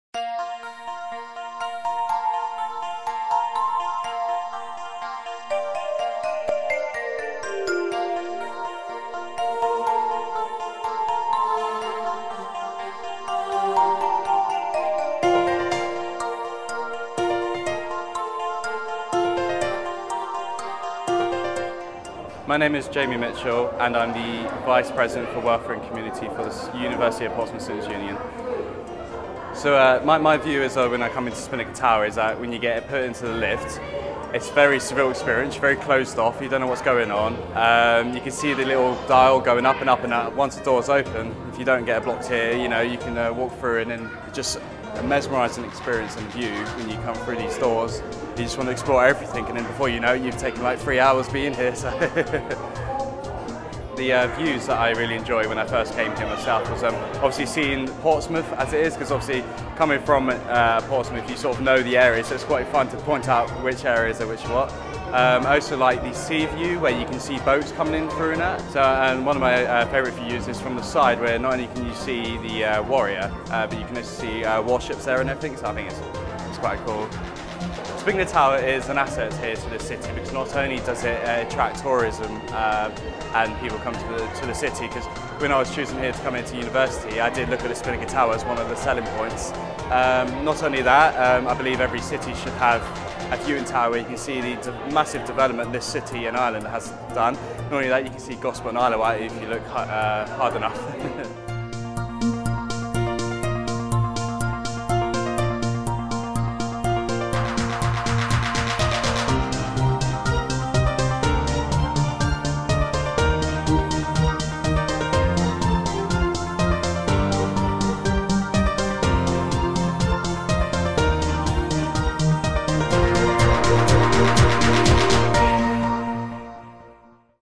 View Things Differently with Jamie Mitchell
WIN!

To celebrate the Emirates Spinnaker Tower's 10 year anniversary in 2015, we've created a series of 10 videos with people who play an important part in making Portsmouth the fantastic city that it is. Enjoy 10 different perspectives on Britain's Best View.

Here, Jamie Mitchell, vice president of Welfare and Community at the University of Portsmouth's Student Union shares his favourite part of the Emirates Spinnaker Tower view.